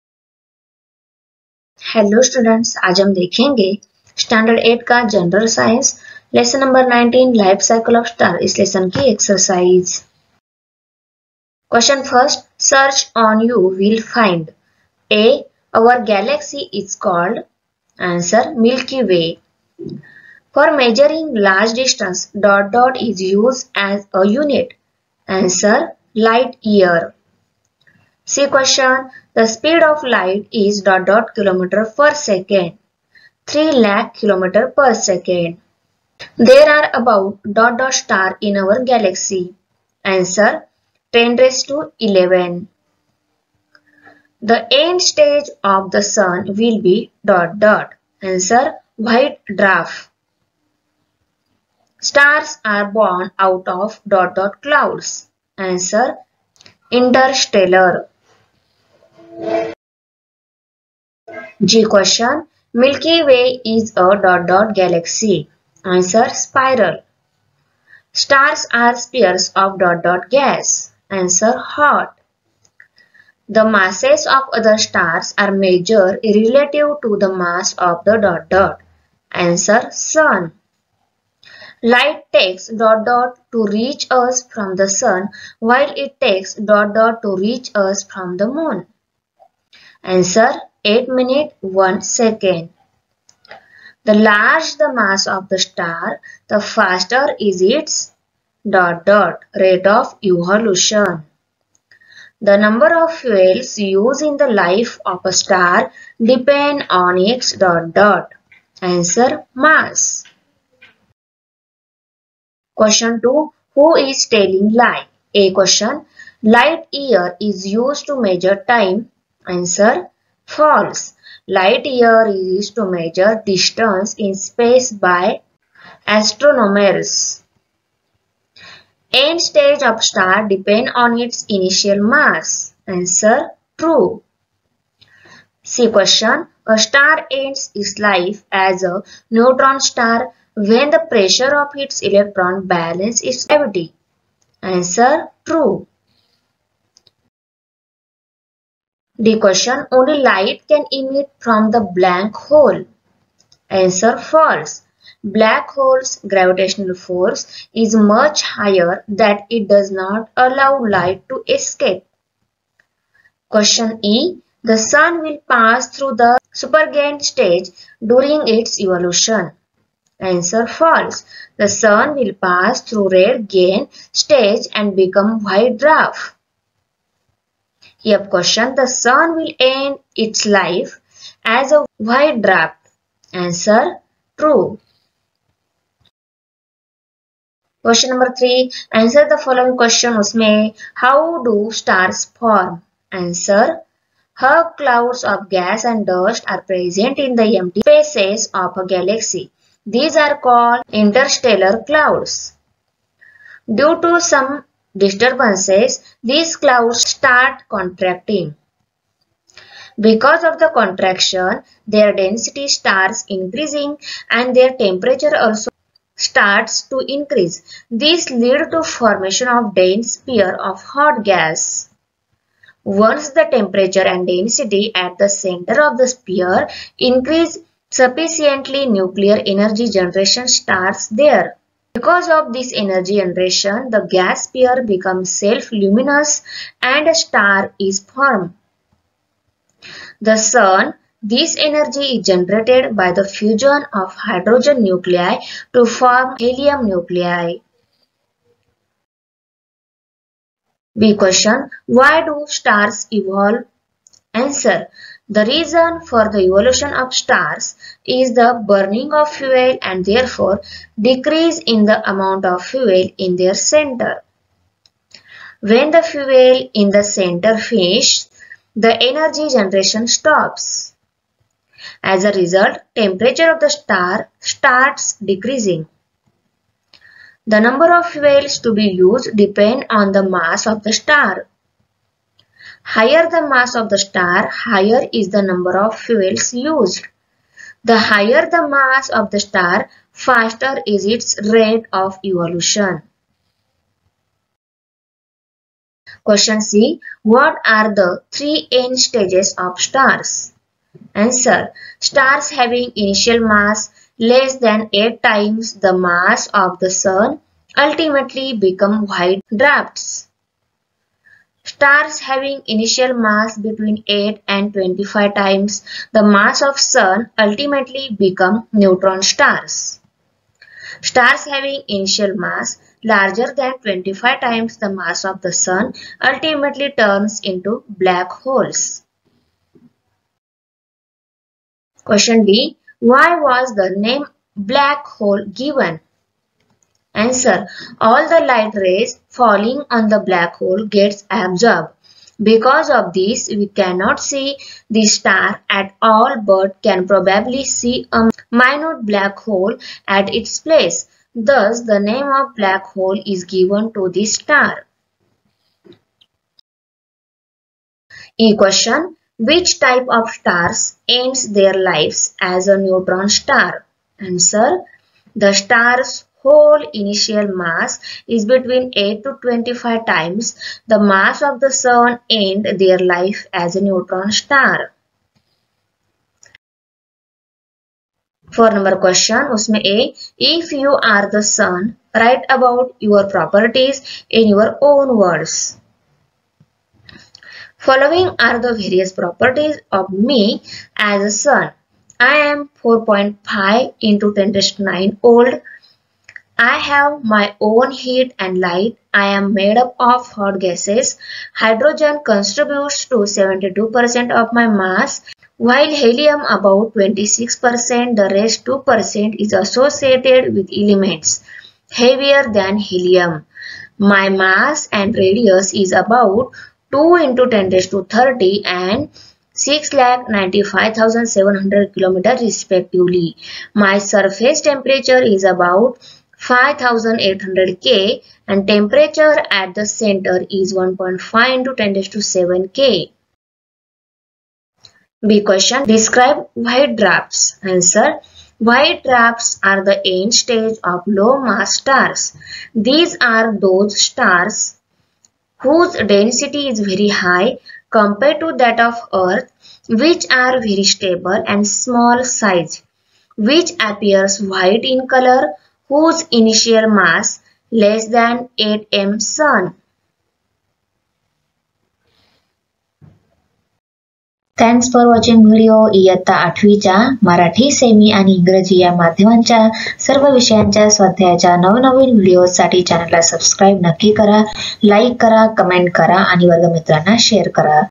Exercise Class 8th Science CH-19 Life cycle of stars Question and Answer | Maharashtra board std 8
Exercise Class 8th Science Life cycle of stars Question and Answer Maharashtra state board std 8 Science